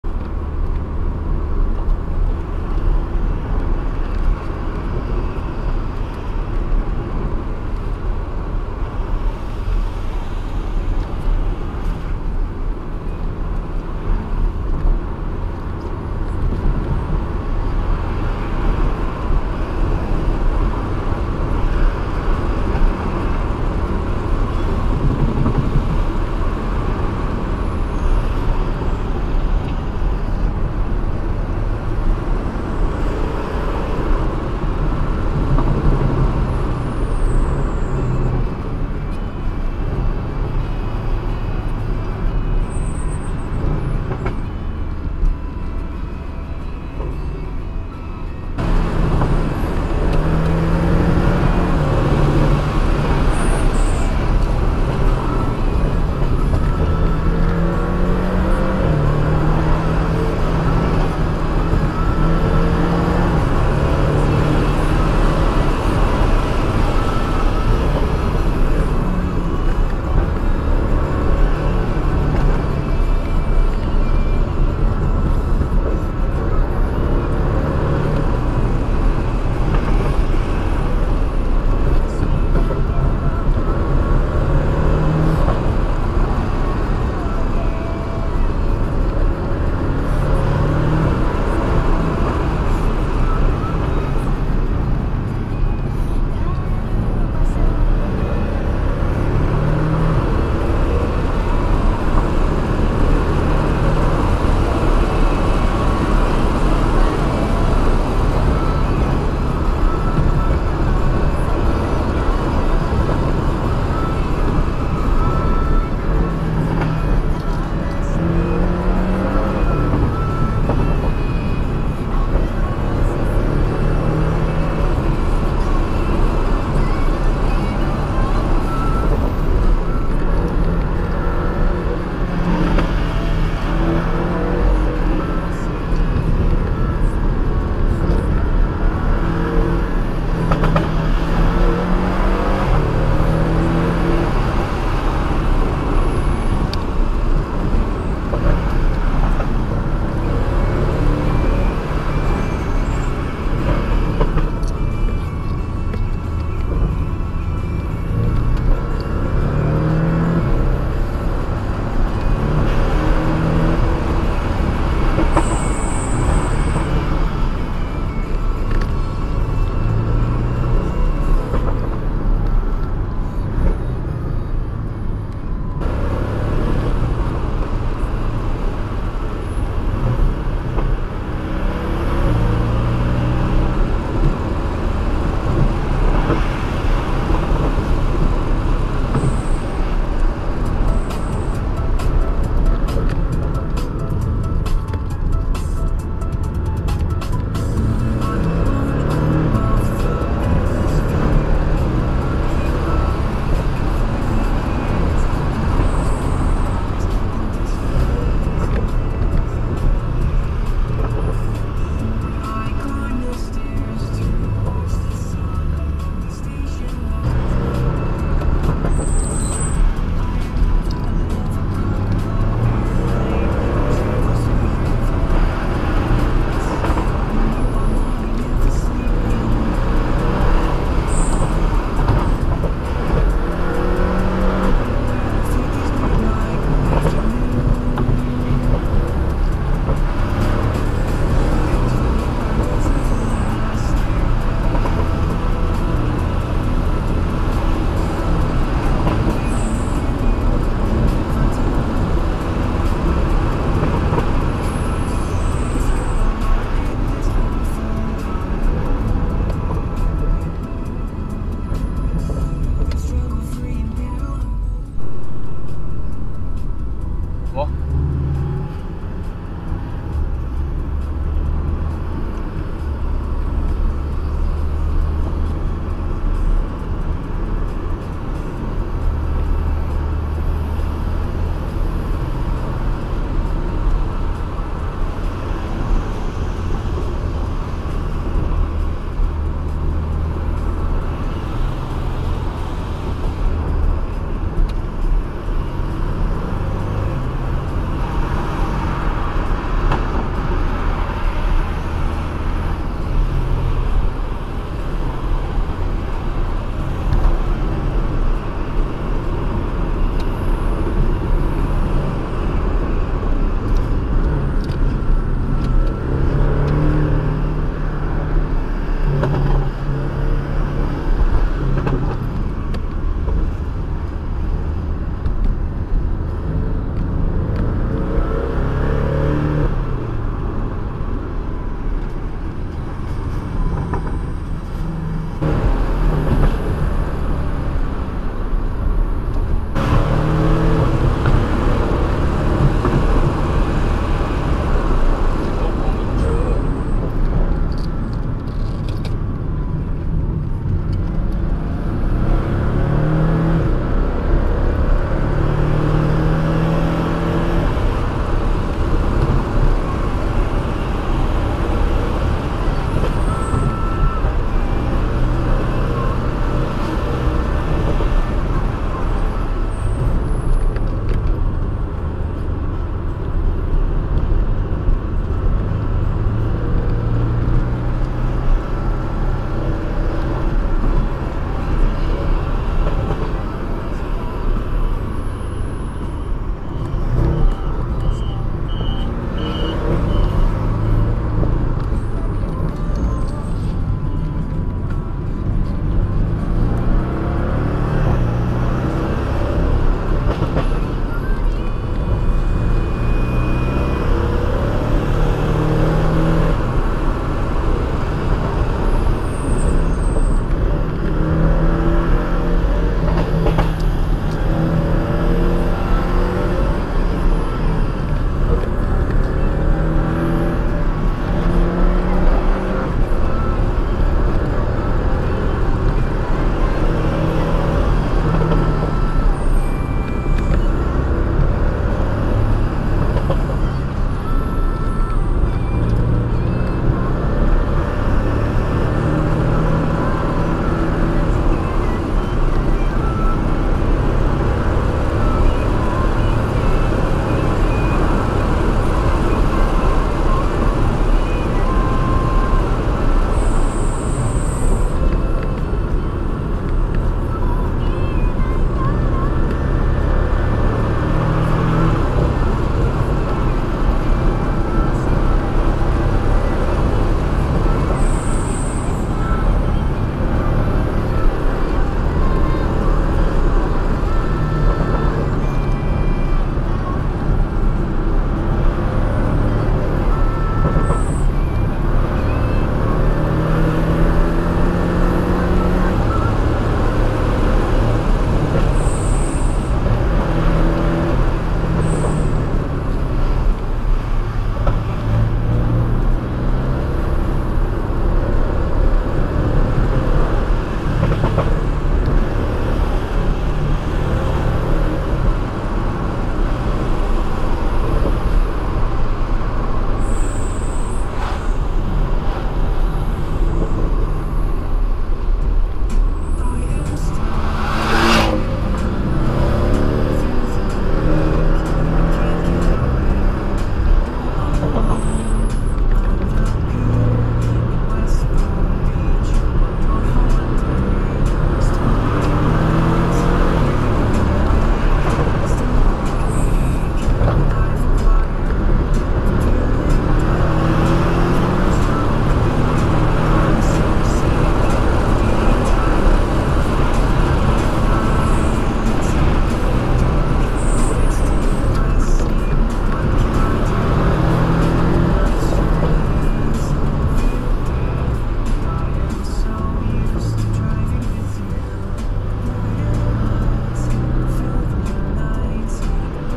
私相賄賂 #982 #718 boxster cayman|HC+ sc7 bbs |0601
在縣界公園等凱宇哥經過,
結果卻發現原來他們出發的比我更早,
早就到山海明珠,
隨即我也趕往山海去。

回程 1起跑,
還遇上國民車cub的集體車聚。